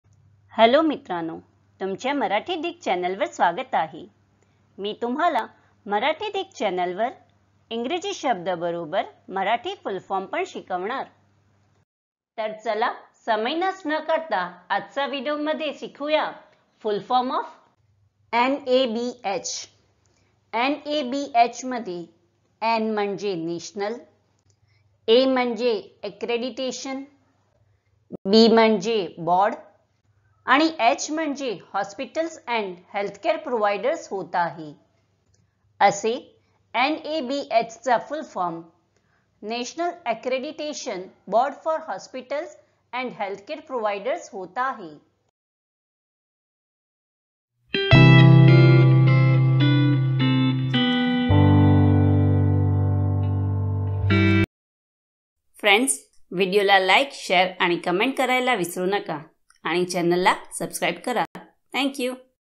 NABH Full Form in Marathi | NABH cha full form kay aahe | NABH म्हणजे काय
या व्हिडिओमध्ये तुम्ही NABHचा पूर्ण फॉर्म मराठीत शिकाल.
NABH चा पूर्ण फॉर्म फक्त एका मिनिटात शिका.